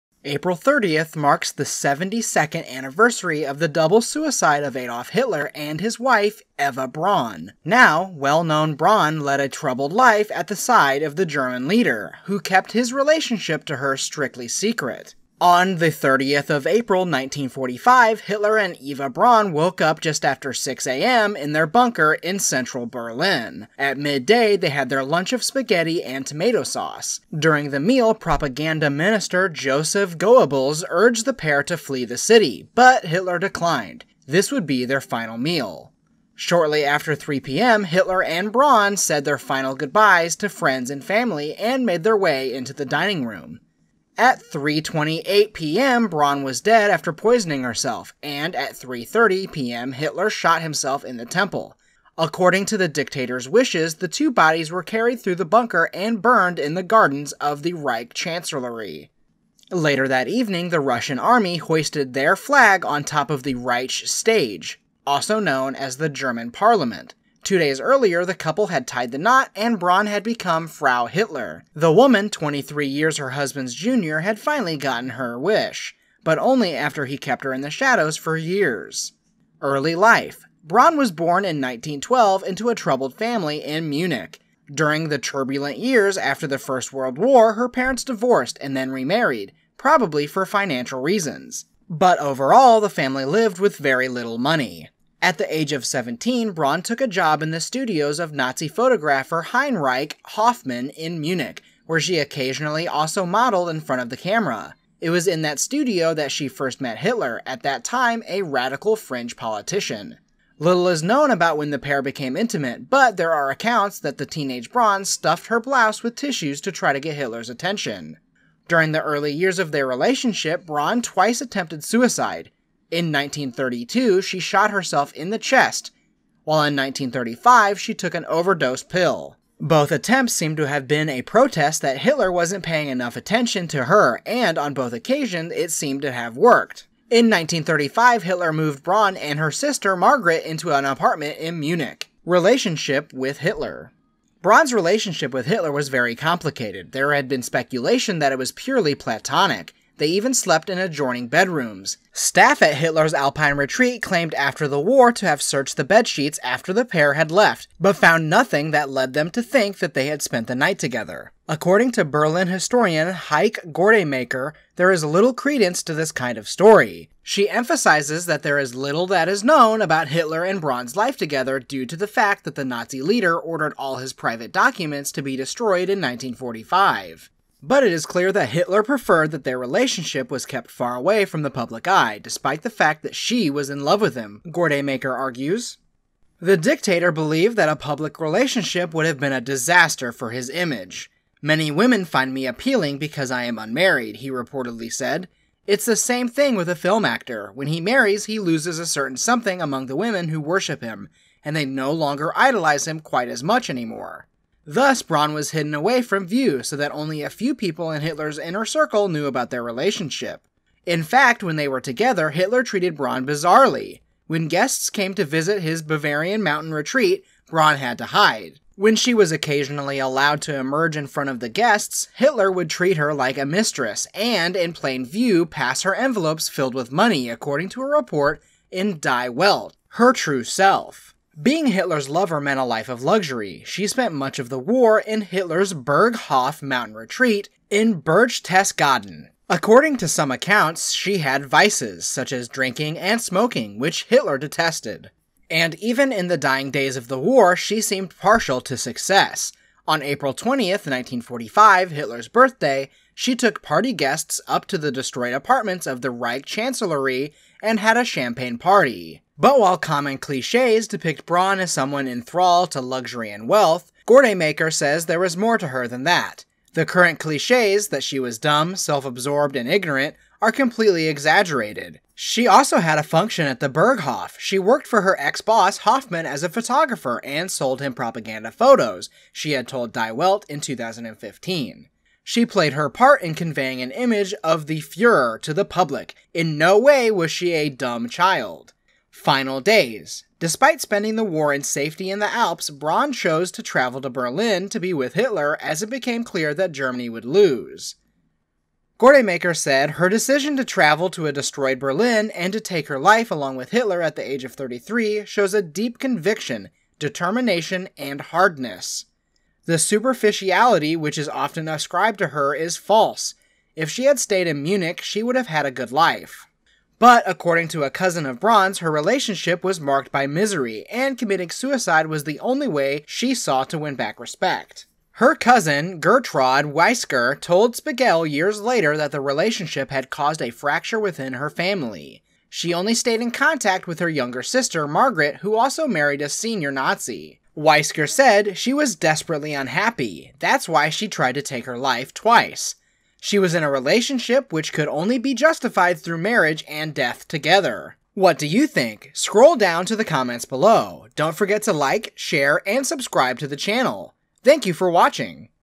The Unhappy Life Of Eva Braun With Adolf Hitler, The Lover Germany Never Knew Hitler Had
Eva Braun led a troubled life at the side of the German leader, who kept his relationship to her strictly secret.

Braun’s relationship with Hitler was very complicated. There has been speculation that it was purely platonic; they even slept in adjoining bedrooms. Staff at Hitler’s alpine retreat claimed after the w.ar to have searched the bedsheets after the pair had left, but found nothing that led them to think that they had spent the night together.

According to Berlin historian Heike Görtemaker, there is little credence to this kind of story. She emphasizes that there is little that is know about Hitler and Braun's life together due to the fact that the Nazi leader ordered all his private documents to be destroyed in 1945.

But it is clear that Hitler preferred it that their relationship was kept far from the public eye, despite the fact that she was in love with him, Görtemaker argues.

The dictator believed that a public relationship would have been a d.isaster for his image.